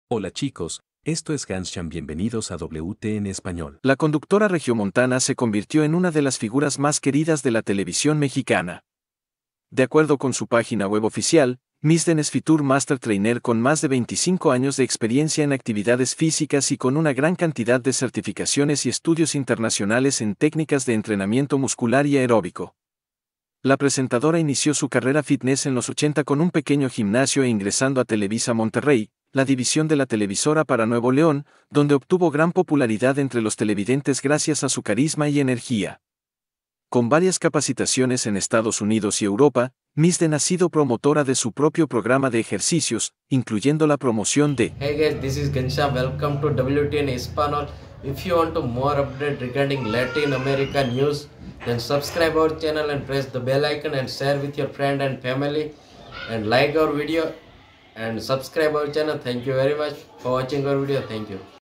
Quién fue y de qué murió Misden Elizondo
La conductora regiomontana se convirtió en una de las figuras más queridas de la televisión mexicana.

De acuerdo con su página web oficial, Misden es Fitour Master Trainer con más de 25 años de experiencia en actividades físicas y con una gran cantidad de certificaciones y estudios internacionales en técnicas de entrenamiento muscular y aeróbico.

La presentadora inició su carrera fitness en los 80 con un pequeño gimnasio e ingresando a Televisa Monterrey, la división de la televisora para Nuevo León, donde obtuvo gran popularidad entre los televidentes gracias a su carisma y energía.

Con varias capacitaciones en Estados Unidos y Europa, Misden ha sido promotora de su propio programa de ejercicios, incluyendo la promoción de